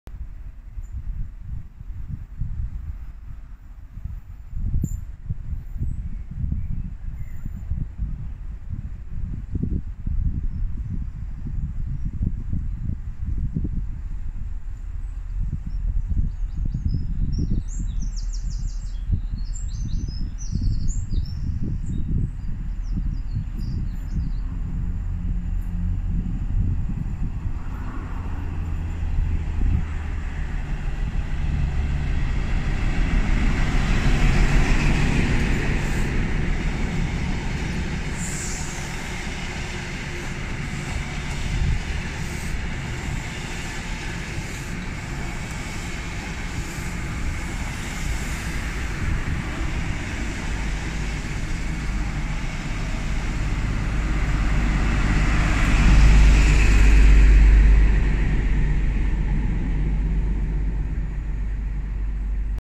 Midland Pullman HST 43046 & 055 at Bermuda Park, Bedworth 12/06/21
Midland Pullman 43046 & 055 pass Bermuda Park, Bedworth with the return Carlisle - Bristol TM.

Filmed Saturday 12th June 2021.